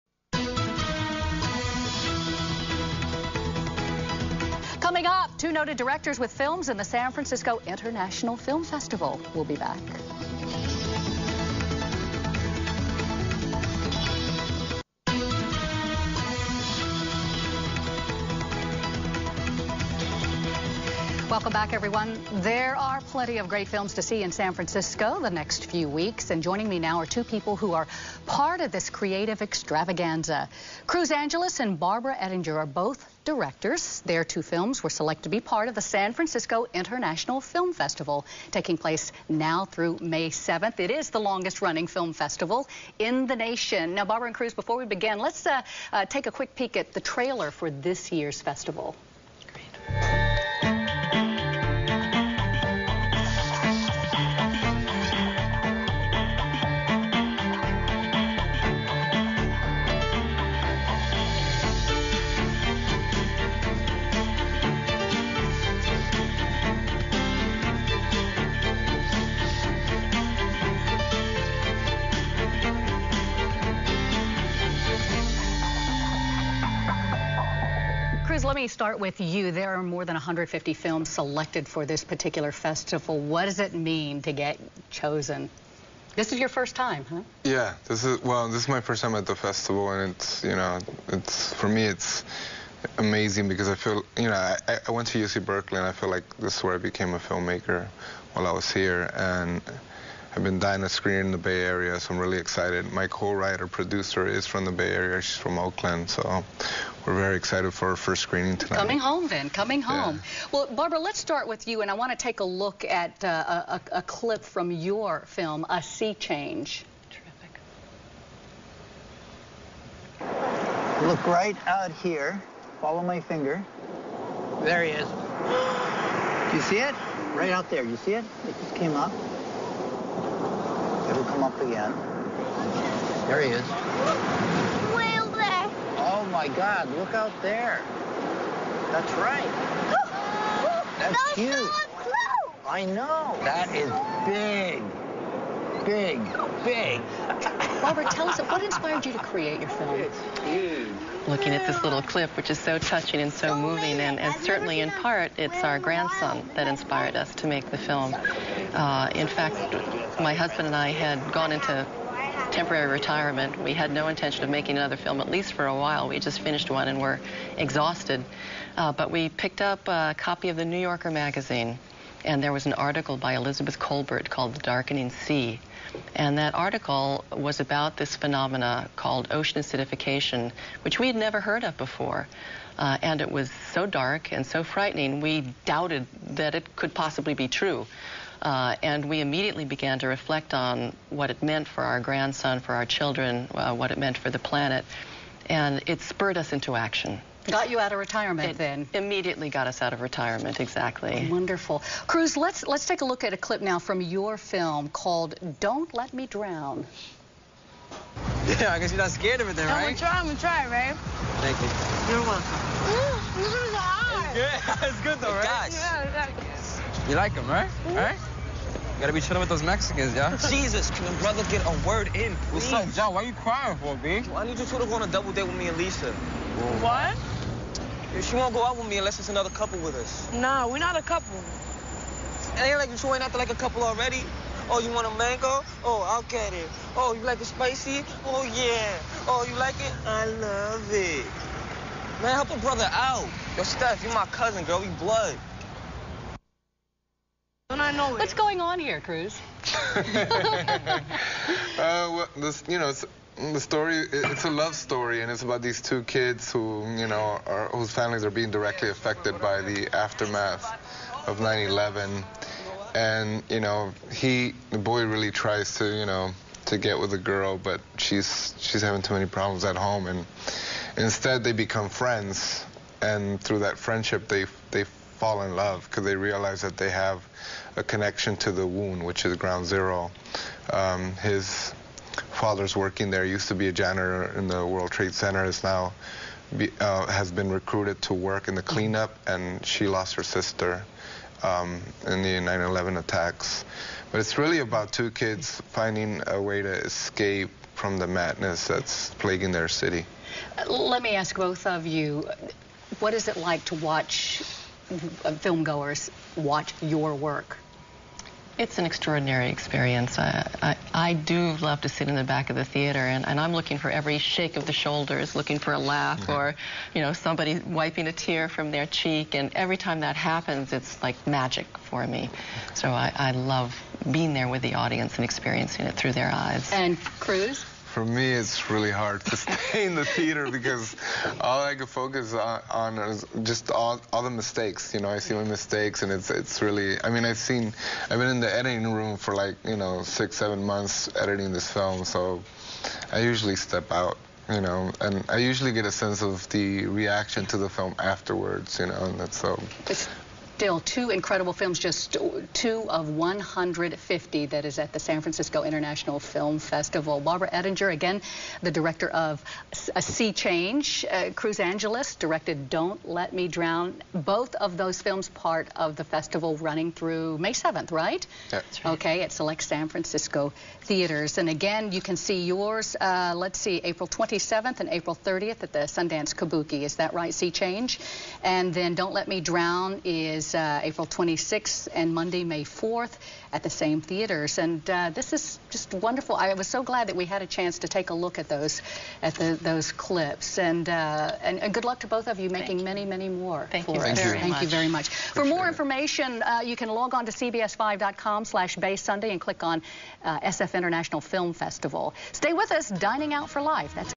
KPIX CBS5 Bay Covers A Sea Change & Don't Let Me Drown
Barbara Ettinger and Cruz Angeles interviewed on KPIX, CBS5 before the San Francisco International Film Festival.